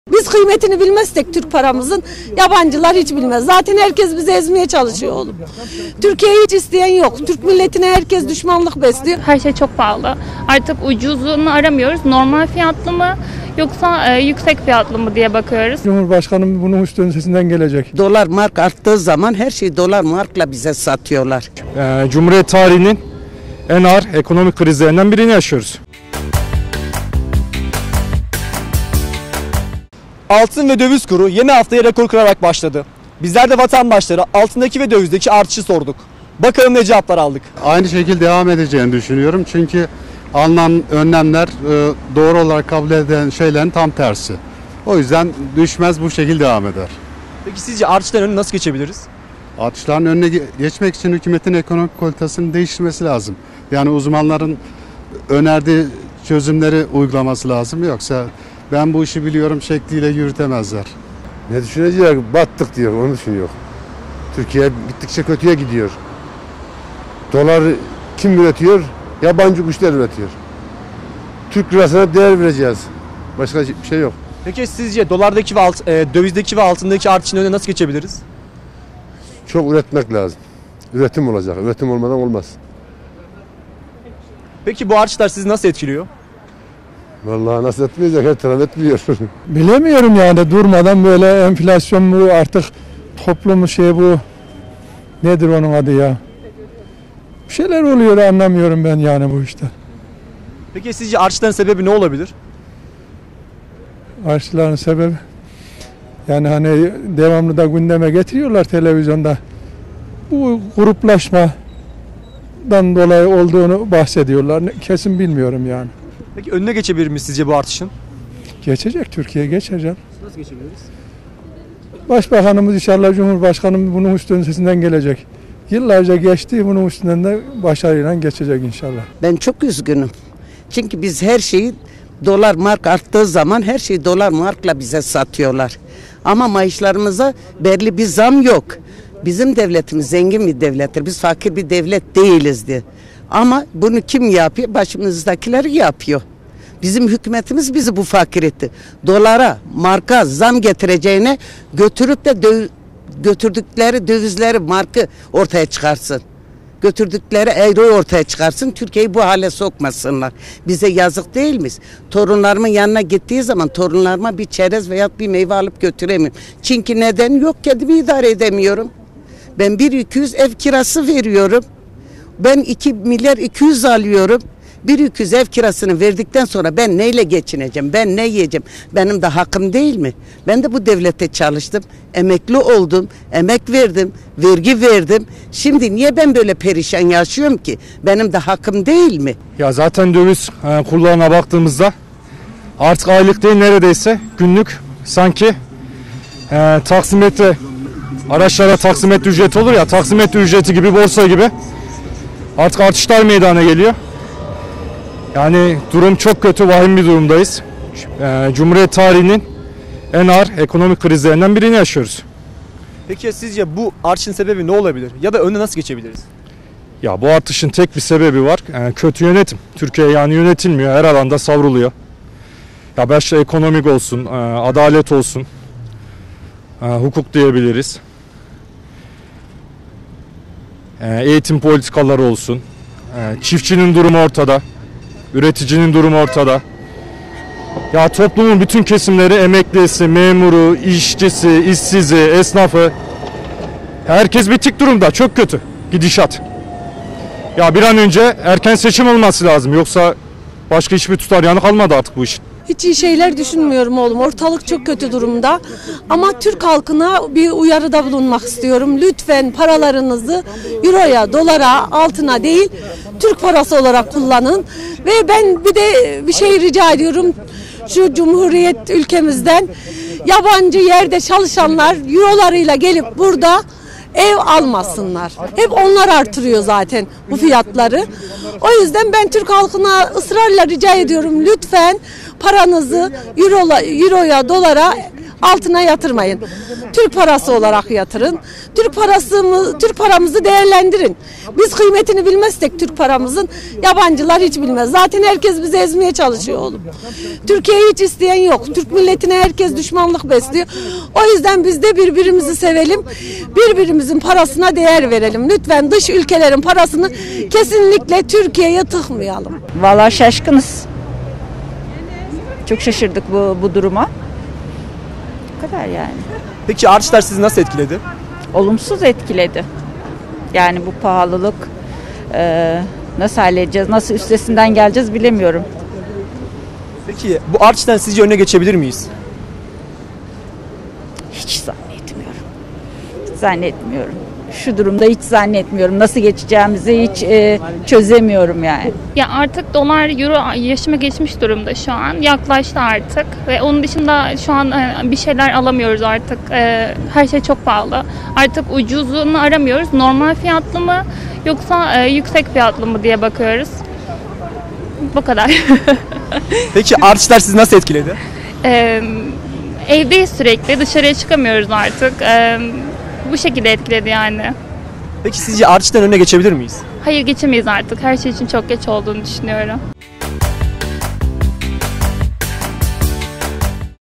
DÖVİZ KURLARI HAFTAYA REKORLA BAŞLADI
Son zamanlarda döviz ve altın kurunda görülen dalgalanma sürüyor. Dolar haftaya 17 lira 23 kuruşla, Euro 19 lira 37 kuruş, gram altın 995 lira ve çeyrek altın ise 1675 liradan başladı.  Kayserim.net Laf Sokakta ekibi sokağa altın ve döviz kurundaki artış  hakkında ne düşünüyorsunuz ? diye sordu.